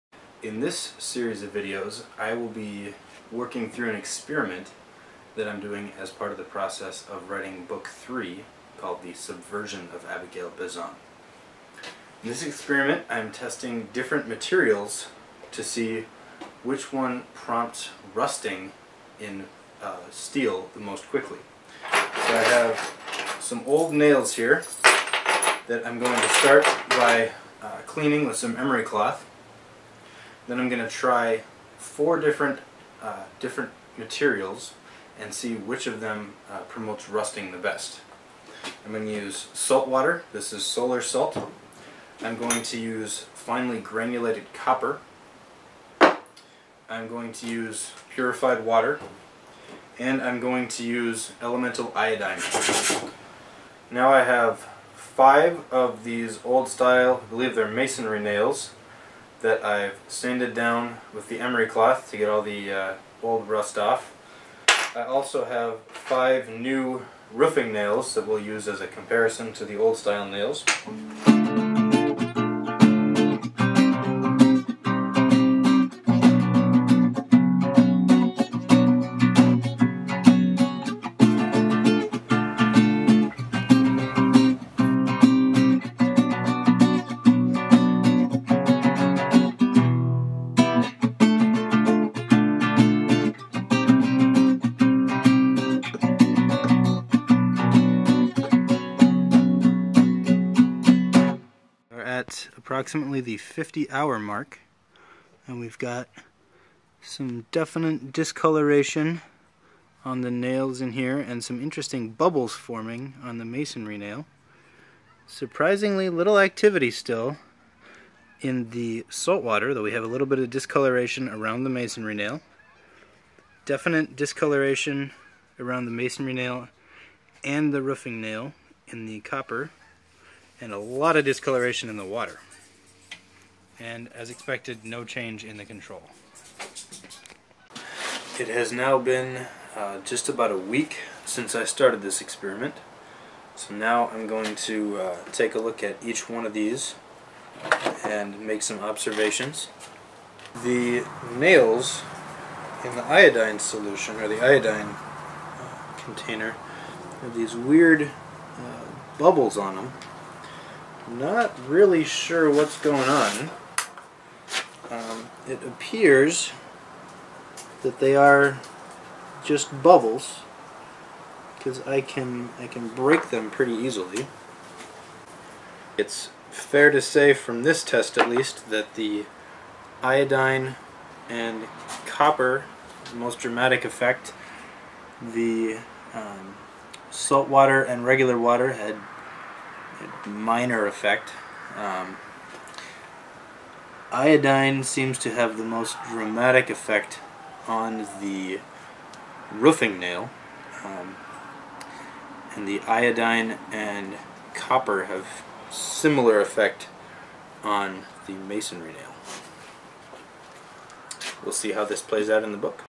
Abigail Beson Rust Experiment
Experiment I conducted as part of background research for "The Subversion of Abigail Beson"